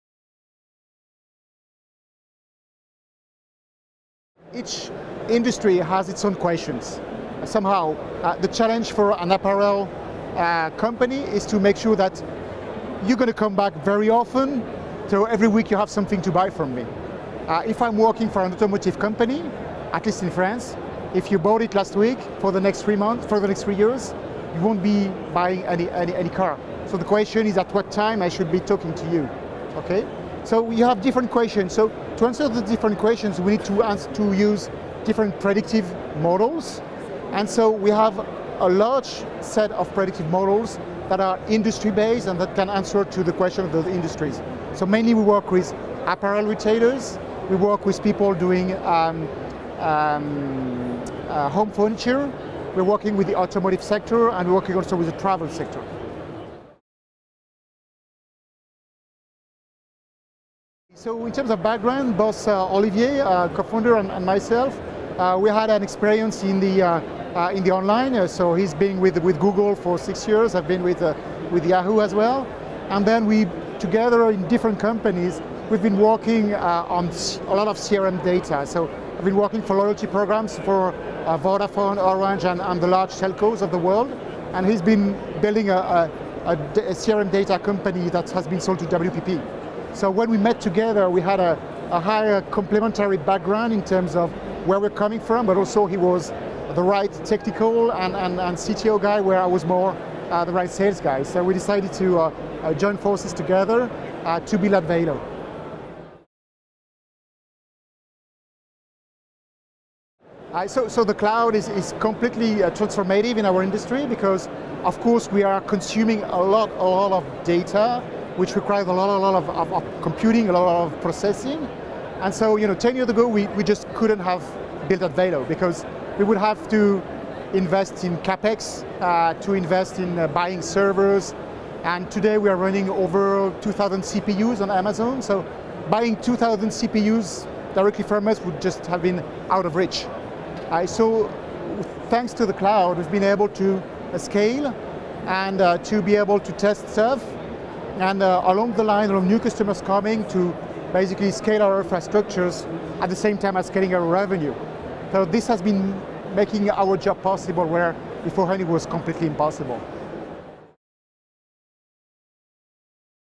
Modèles prédictifs et données CRM : Interview de David Le Douarin, co-fondateur d'Advalo
Interview de David Le Douardin, co-fondateur et Directeur général d'Advalo, lors du salon Viva Technology.

Il vous explique en quoi consiste la plateforme de marketing prédictif et revient sur son expérience professionnelle dans les données et la relation client, ainsi que celle d'Olivier Marc, également co-fondateur de l'entreprise.

Enfin, il vous explique comment AWS a contribué au succès et à la la montée en puissance de la pépite de la French tech.